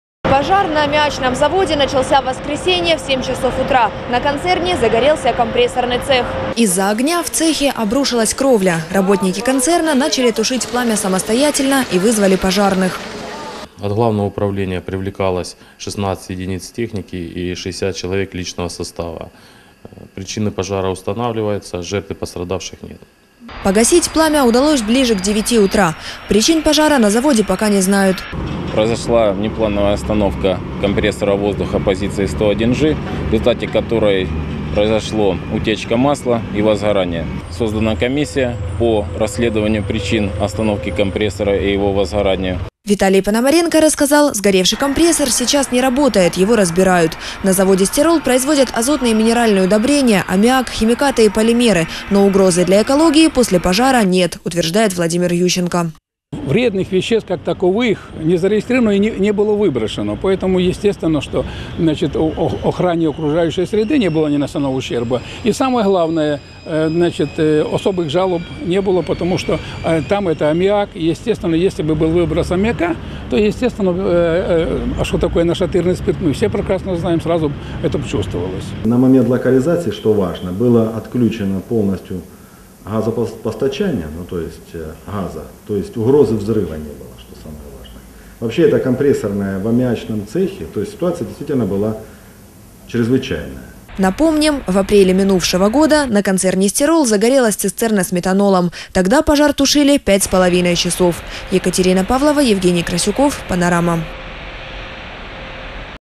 На Стироле произошел пожар
Погибших  и раненных нет. На химическом концерне «Стирол» в Горловке произошел пожар. Огонь тушили больше полусотни пожарных. Подробнее о происшествии расскажет Екатерина Павлова.